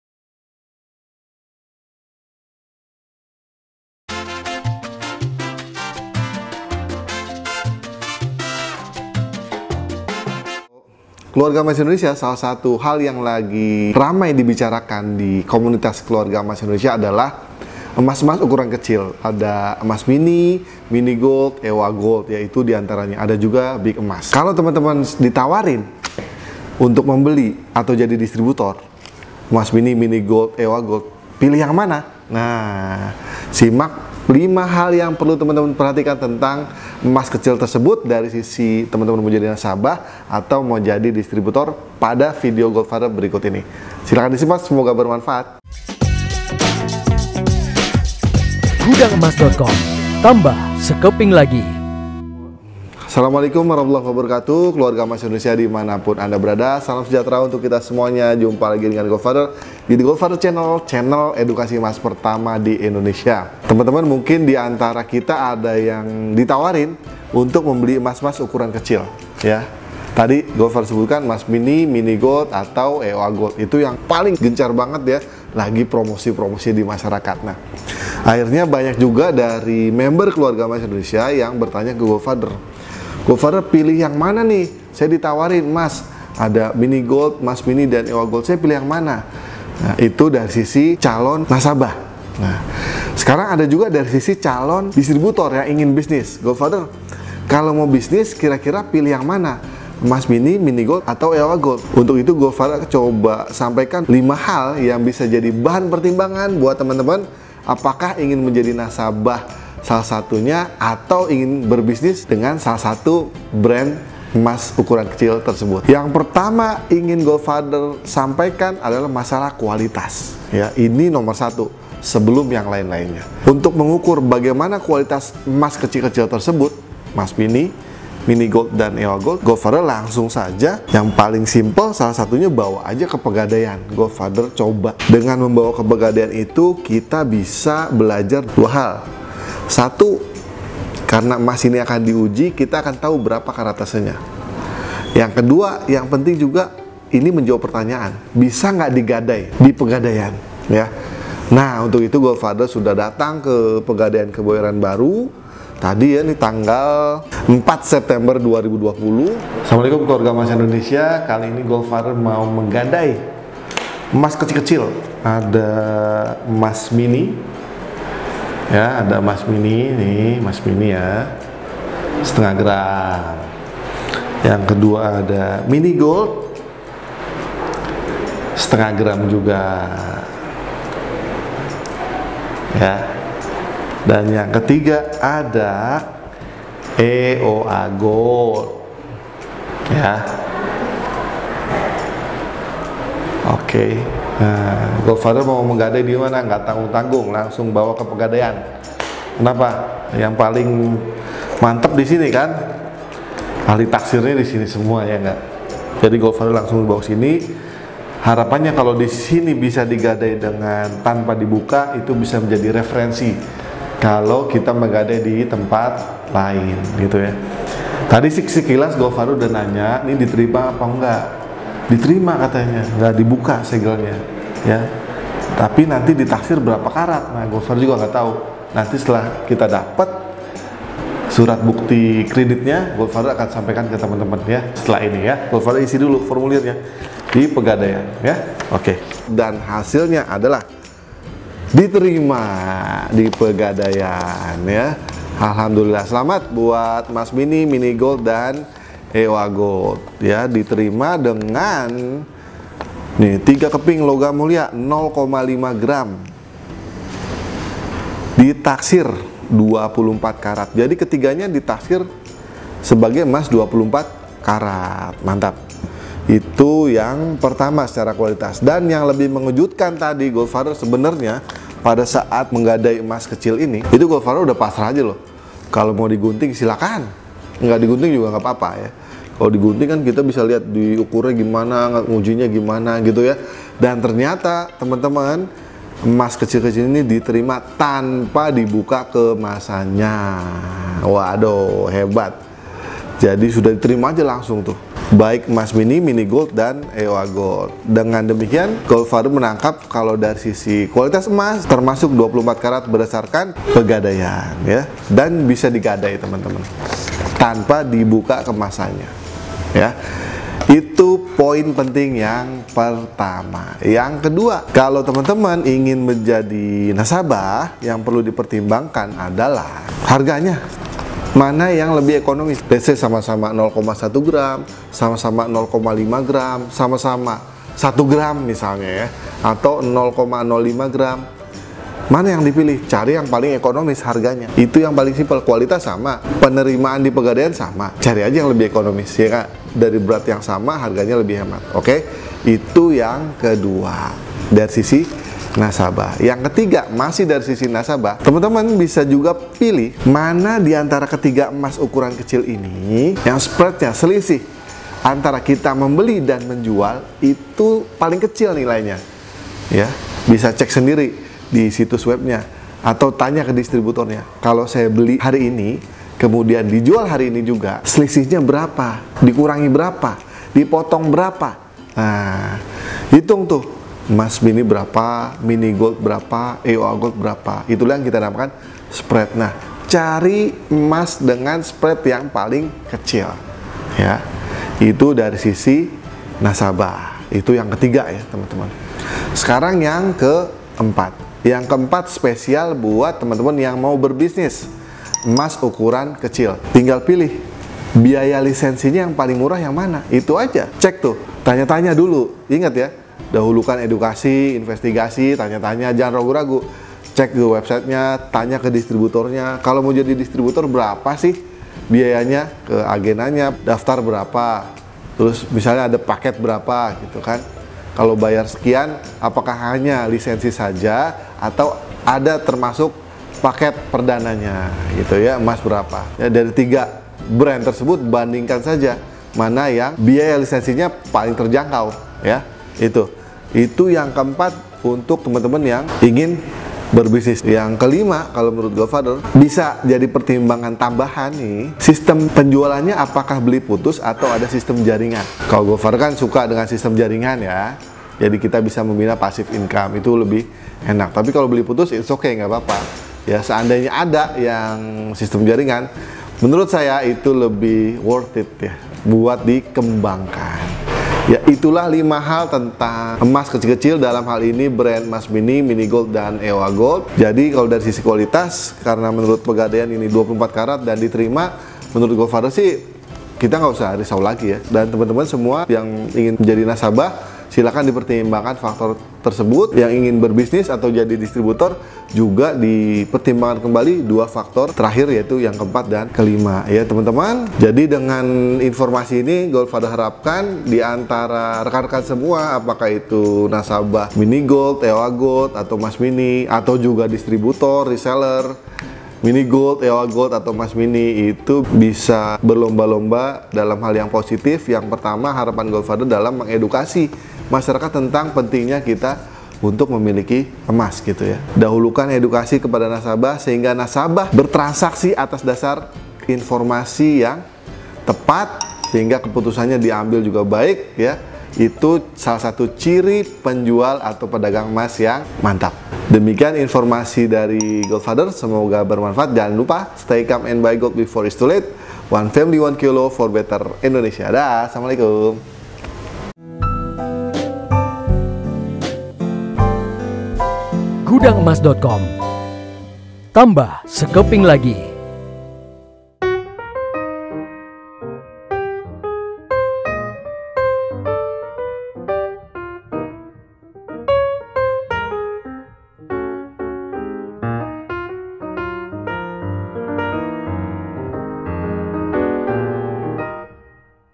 Emas Mini, Mini Gold dan EOA GOLD, Mana yang Paling Oke nih Gaes?
Banyak Pertanyaan ke goldfather terkait emas ukuran kecil terutama dari Brand EmasMini MiniGold dan juga

seperti, itu emasnya asli apa nggak ya gf?
atau, kalau mau bisnis pilih yg mana gf?

nah akhirnya goldfather buat lagi 1 video khusus sebagai referensi bagi rekan-rekan semuanya.
semoga bisa menjawab kegelisahan rekan-rekan semuanya ya.

Stay Calm & Buy

1 Family
1 Kilo
4 Better Indonesia